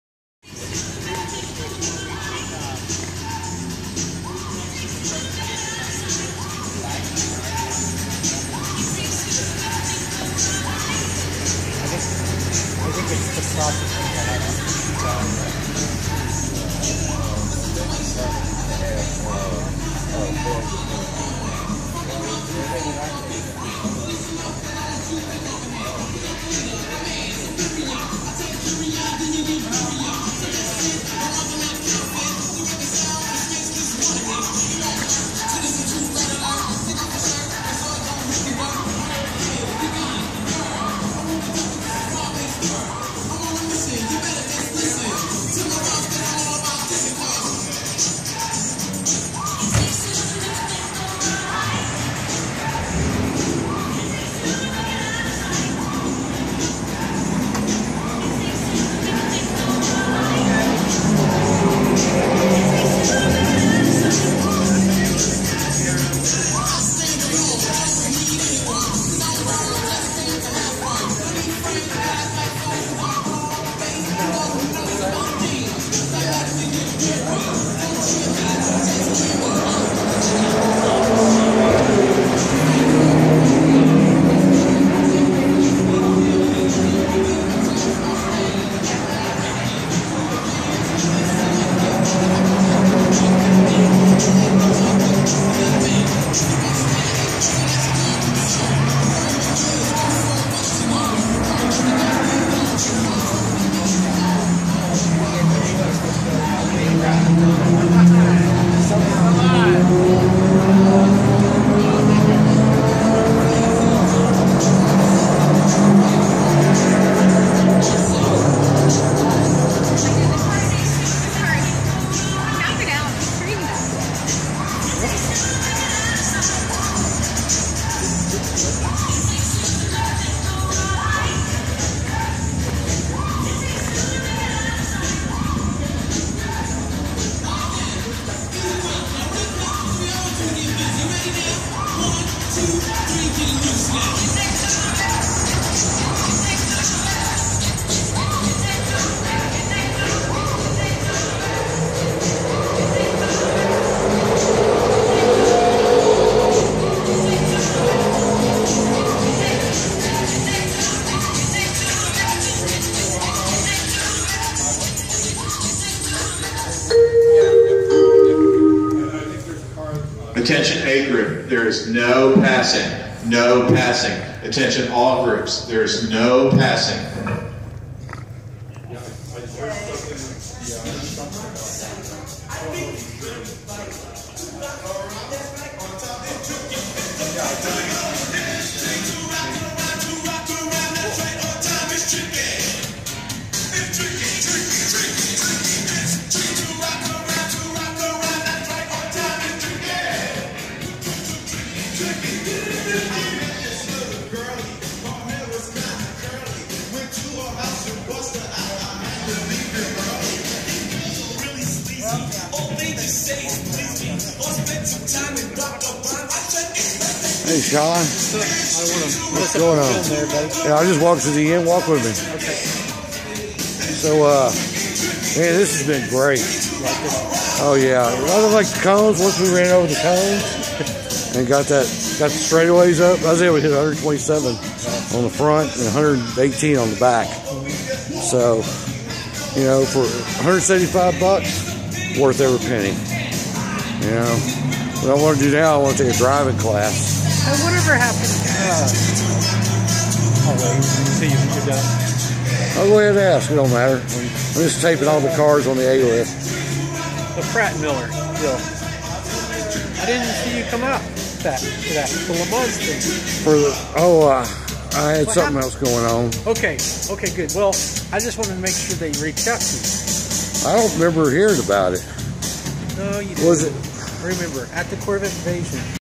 COTA track day. Cars in group A, 1/10/16
Walk by of group A cars.